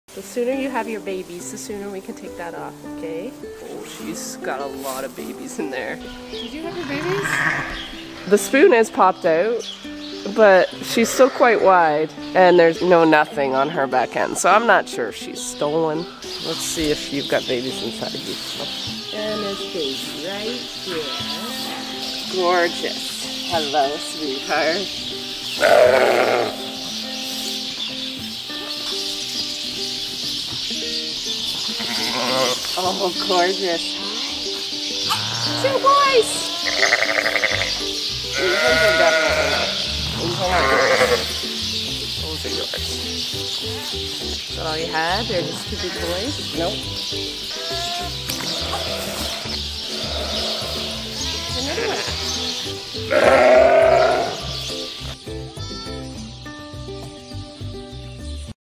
MAMA!!! DID YOU HAVE YOUR BABIES??? 😱 ...time to take this harness OFF!
Our prolapse ewe is in labour!!!